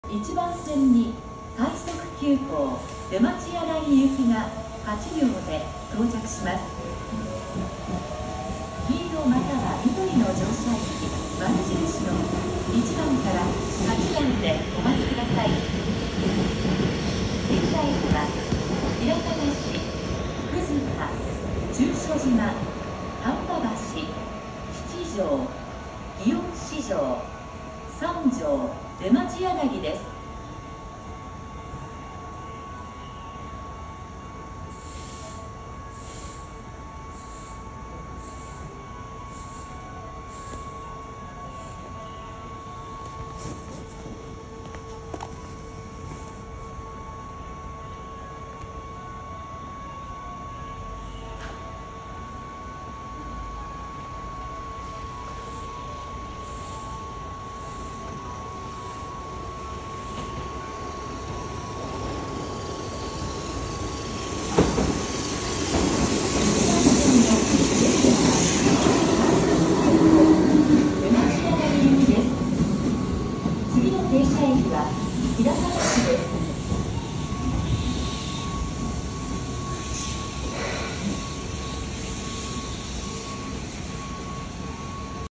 京阪香里園駅6000系快速急行出町柳行き到着
この後の4分後の特急に追いつかれるが抜かされることのない意味わからんようなダイヤ組み合わせにしたダイヤ
昔は樟葉止めでそこからの特急連絡だったのが今なればかなりの確率で減ってきてるがロング快速急行が大復活しつつある。